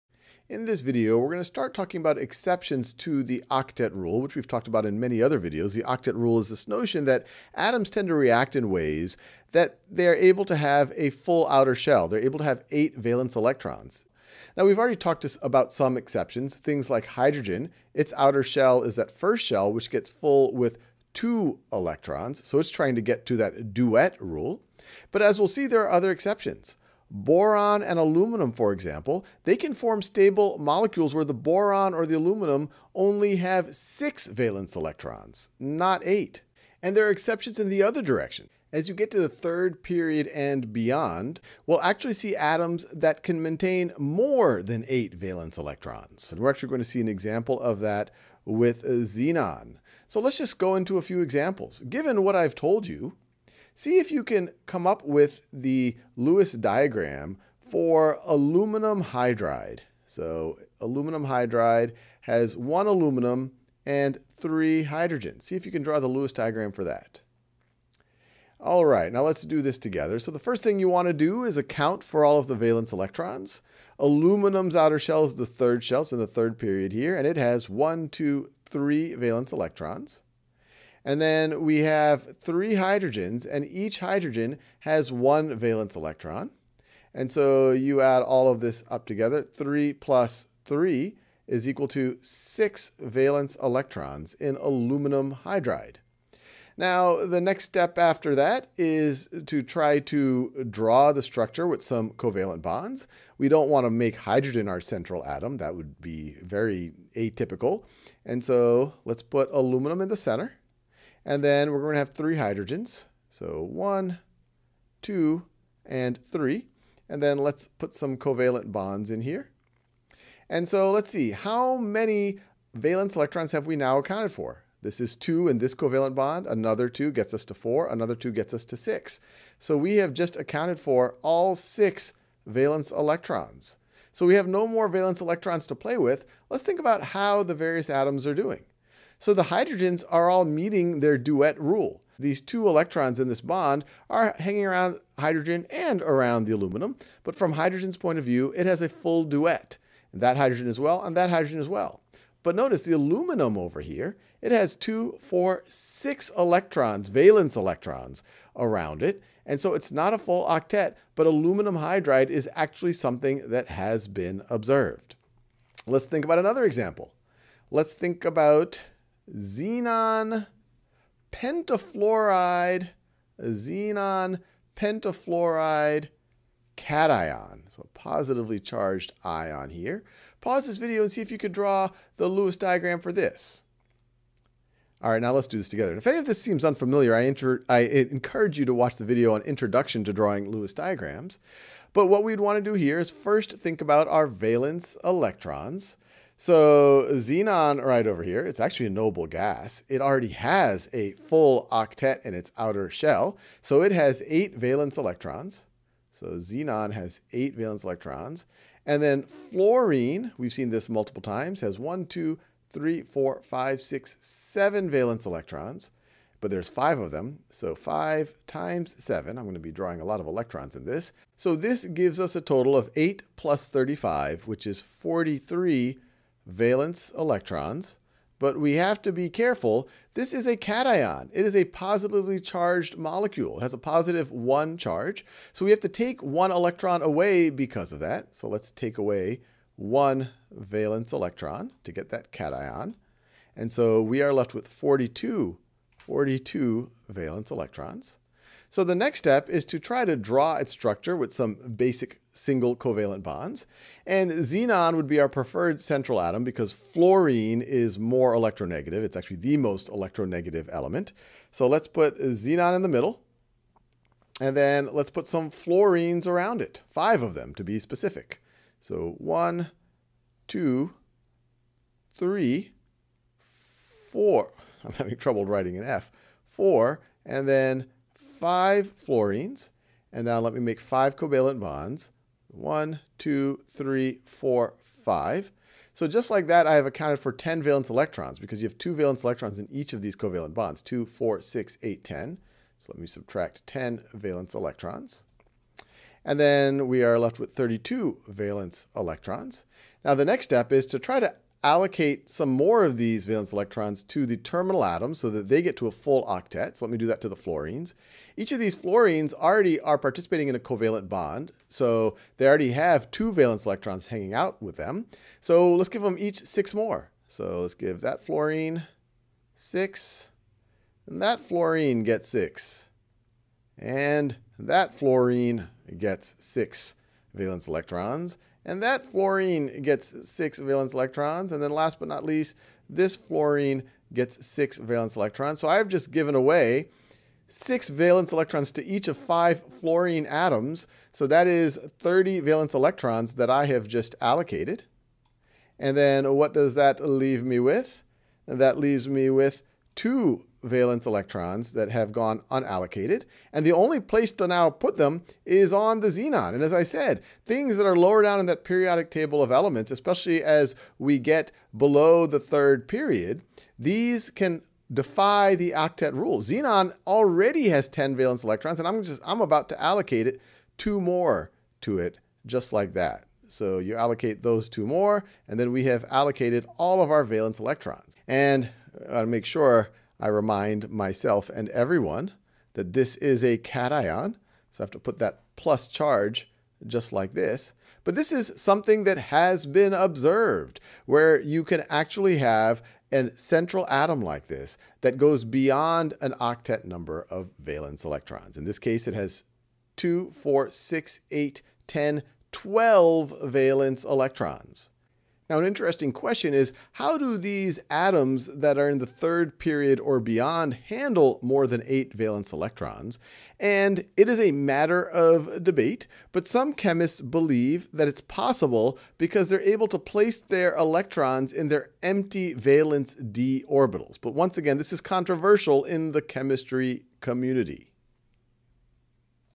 Exceptions to the octet rule | AP Chemistry | Khan Academy
While most atoms obey the duet and octet rules, there are some exceptions. For example, elements such as boron or beryllium often form compounds in which the central atom is surrounded by fewer than eight electrons (e.g., BF₃ or BeH₂). In contrast, many elements in the third-row and beyond have been observed to exceed the octet rule, forming compounds in which the central atom is surrounded by more than eight electrons (e.g. ICl₄⁻ or PF₅). View more lessons or practice this subject